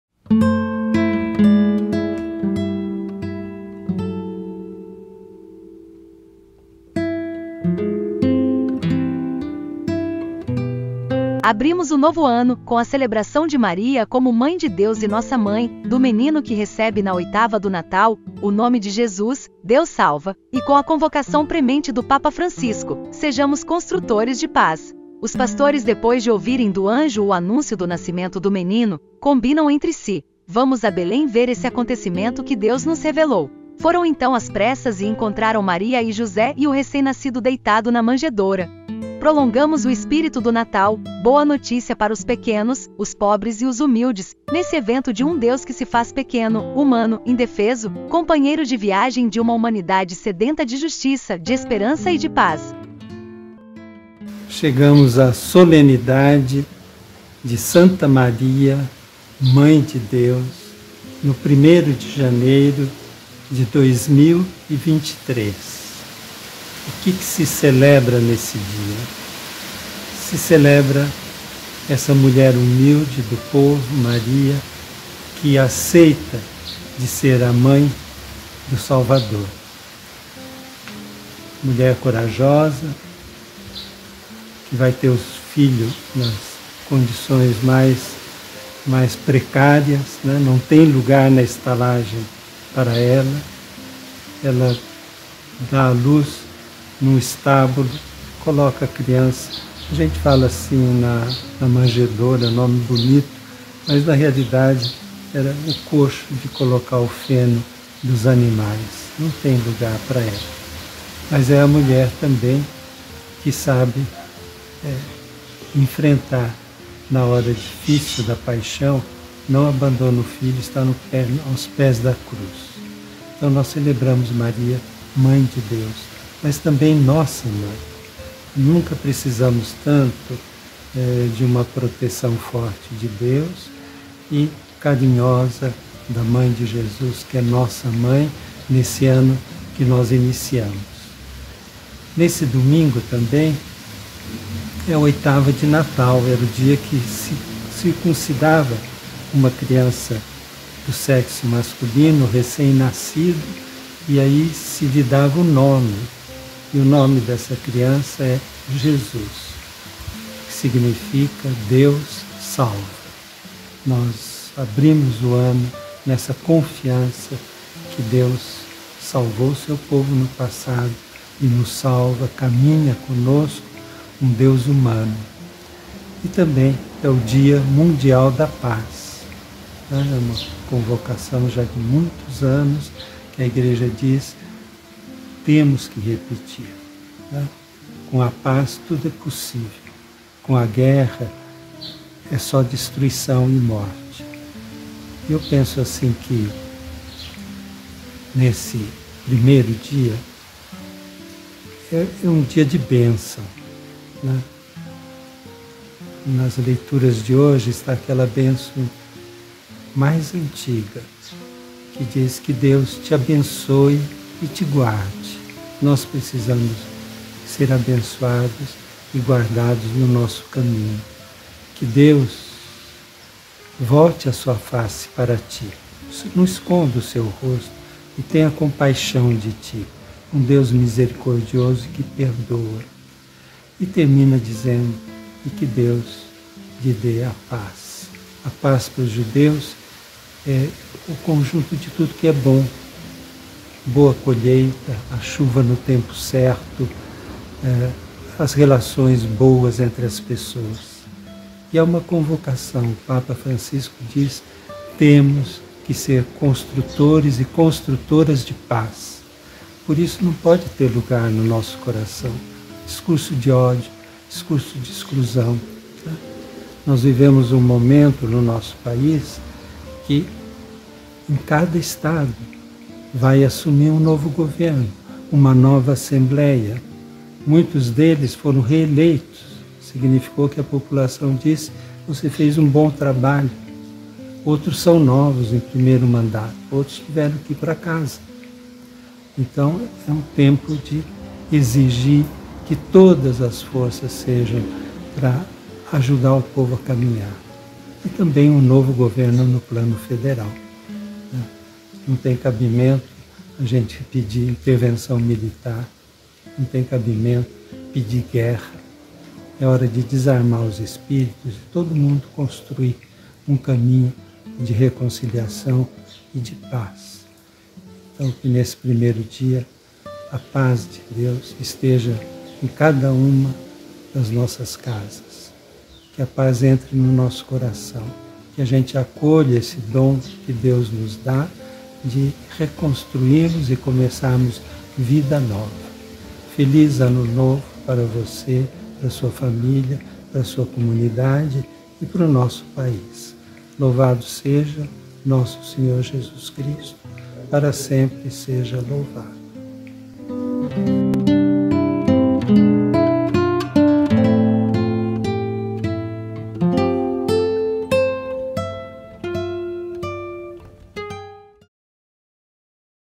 PADRE BEOZZO: Solenidade de Santa Maria, Mãe de Deus e Dia mundial da Paz (Lc 2, 6-21)
Abrimos o novo ano, com a celebração de Maria, como Mãe de Deus e nossa Mãe; do menino que recebe na oitava do Natal, o nome de Jesus, “Deus salva” e com a convocação premente do Papa Francisco: “Sejamos construtores/as de paz”.

Os pastores depois de ouvirem do anjo o anúncio do nascimento do menino, combinam entre si:
“Vamos a Belém ver esse acontecimento que Deus nos revelou. Foram então às pressas e encontraram Maria e José e o recém-nascido deitado na manjedoura”.
Prolongamos o espírito do Natal, boa notícia para os pequenos, os pobres e os humildes, nesse evento de um Deus que se faz pequeno, humano, indefeso, companheiro de viagem de uma humanidade sedenta de justiça, de esperança e de paz.